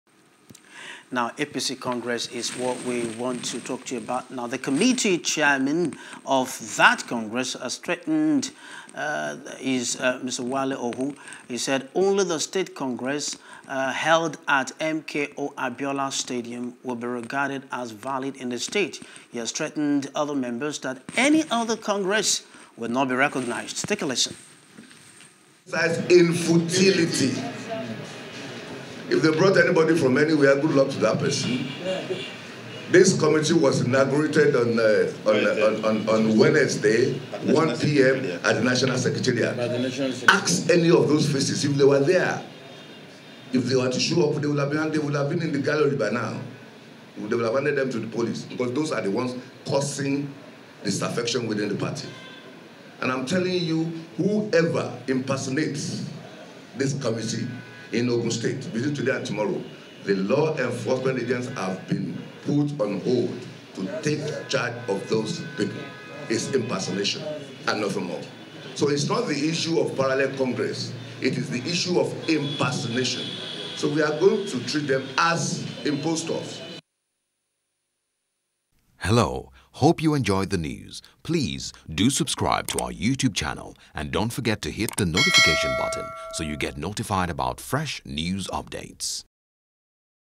Committee Chairman Threatens Imposter In Ogun | NEWS
Ogun state Chairman Congress Committee of  A.P.C Wale Ohu has said only the state congress held at M.K.O Abiola Stadium will be regarded as valid in the state.